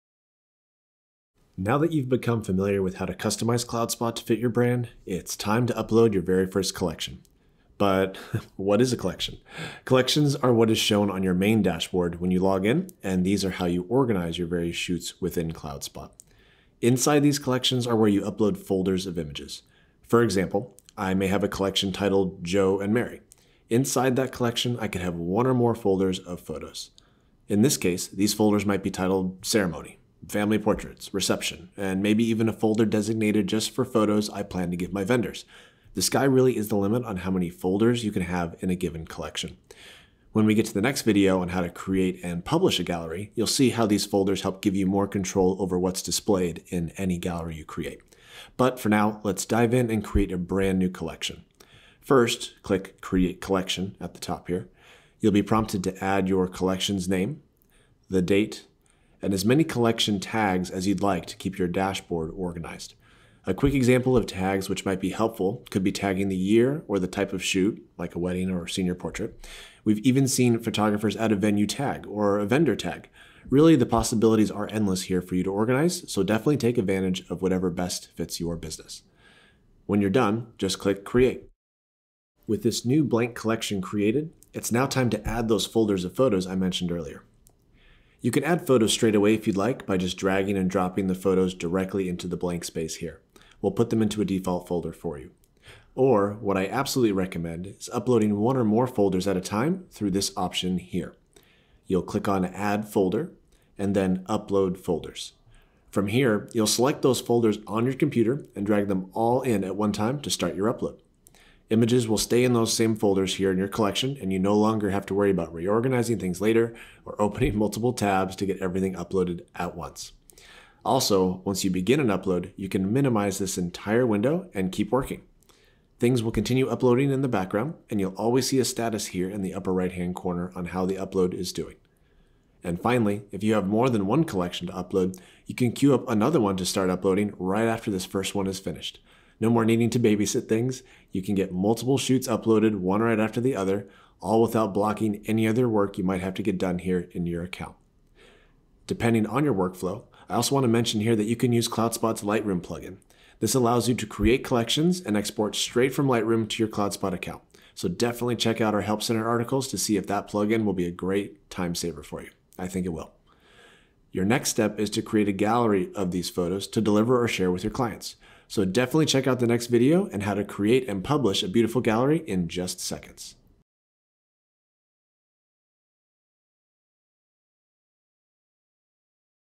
Creating Collections and Uploading Images to CloudSpot
Learn how to organize your images and streamline uploading folders of photos within CloudSpot!